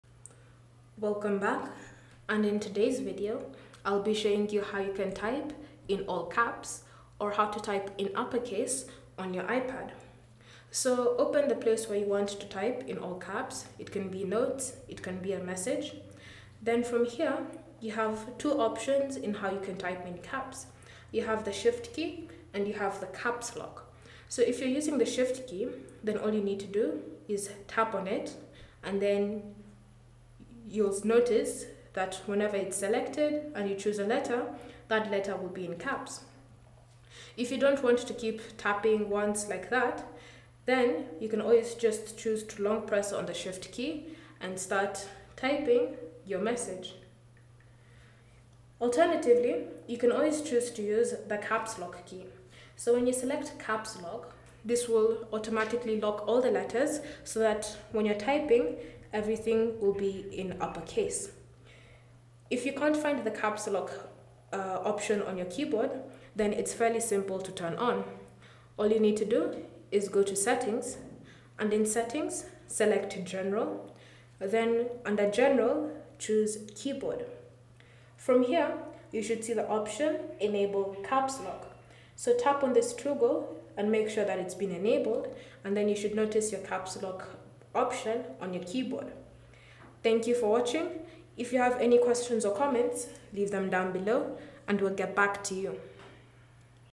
How To Type In All Caps On iPad
In this tutorial, we'll show you how to type in all caps on your iPad. Use different methods and keyboard settings on any iPad, inclusive of M4 iPad Pro.

Whether you're writing an email, composing a document, or just want to emphasize something in your text, learning these simple steps can save you time and effort.

How To Type In All Caps On M4 iPad Pro:
How To Type In All Caps On iPad Pro:

Method 1: Using the Shift Key

Step 1: Begin by opening any app where you can type text, such as Notes or Mail.

Step 2: By default, the keyboard on your iPad displays lowercase letters. Tap the Shift key located on the left side of the keyboard. This activates the shift key, which will change the keyboard to uppercase letters.

To lock the keyboard in caps lock, double-tap the shift key or simply press and hold on it as you type.

Step 3: With the shift key activated, simply start typing. All letters you type will appear in uppercase.

Step 4: To return to lowercase letters, tap the shift key again or simply tap the shift key after typing your uppercase text.

Method 2: Using Caps Lock

Step 1: Similar to a traditional keyboard, the iPad also allows you to lock the keyboard in uppercase mode.

Step 2:  Tap on the "Caps Lock" key on the keyboard. When caps lock is on, the key will change to a darker shade to indicate it's locked.

Step 3: Once caps lock is enabled, every letter you type will be in uppercase until you turn off caps lock.

Step 4: To turn off caps lock, tap the Caps Lock key once again. It will return to its original color, indicating that caps lock is no longer active.

If you don't see the Caps Lock key on your keyboard, you can add it via Settings. Simply open the Settings app and tap on "General" in the left-hand side panel.

Under General, scroll down and tap on "Keyboard". Here, find the "Enable Caps Lock" option and enable it by tapping on the toggle button next to it.

Cell Phone Tripod Adapter